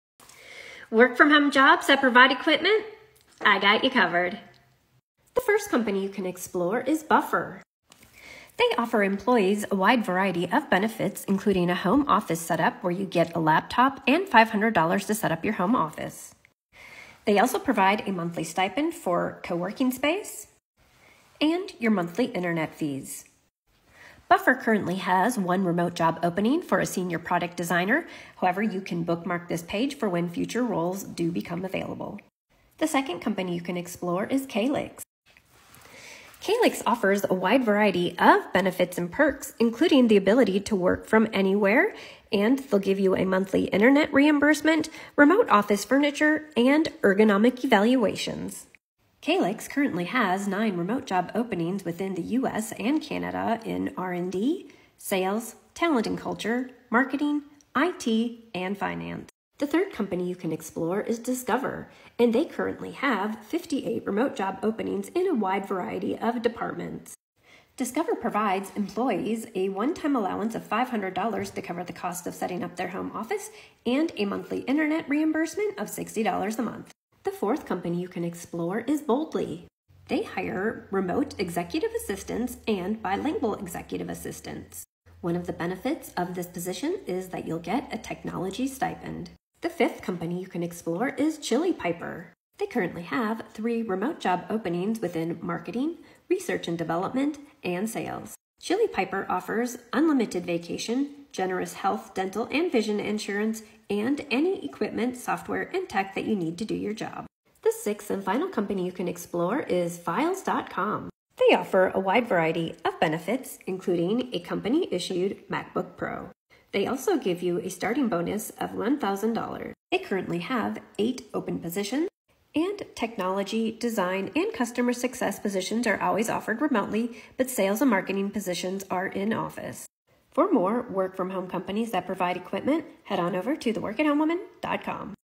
Work From Home Jobs That Provide Equipment
Here are some legit work from home companies that will provide your equipment.

Hi, I'm Holly, a former nurse turned career blogger. Welcome to my videos here on The Work at Home Woman.